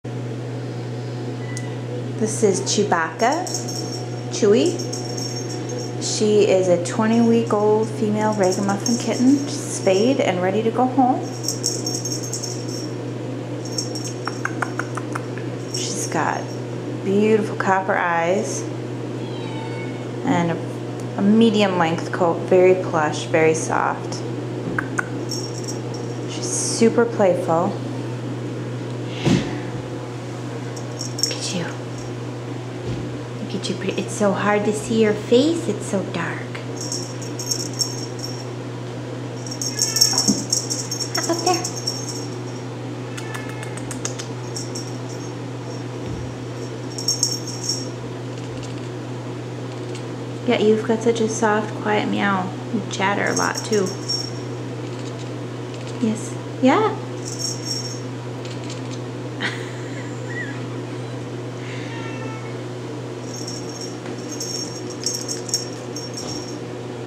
RagaMuffin Kittens-Chewie-Imperial Rags
This is our last kitten available from the Star Wars litter.  for more info.